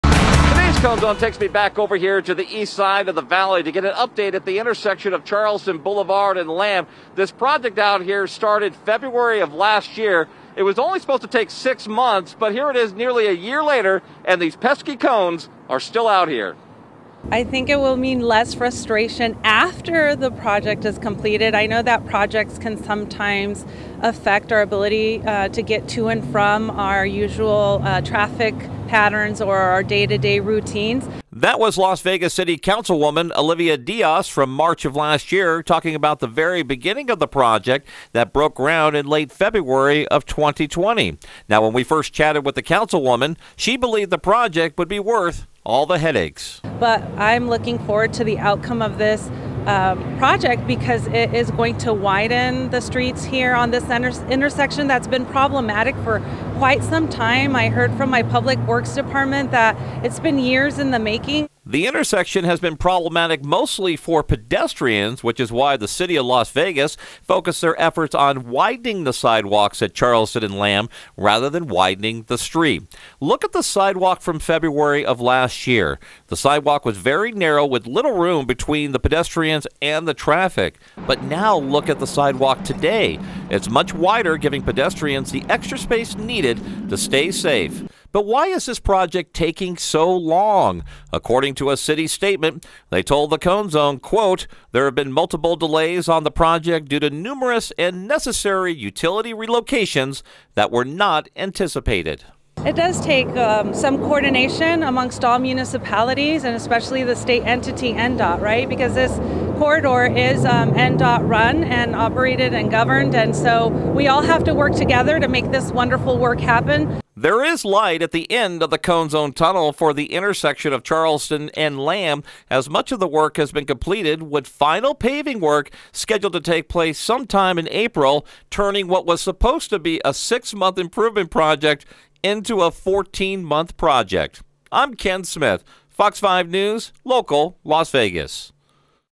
Charleston and Lamb sidewalks expansion project continues
The project was supposed to take just a few months. One year later, residents and pedestrians look forward to wider sidewalks near Charleston and Lamb Boulevard.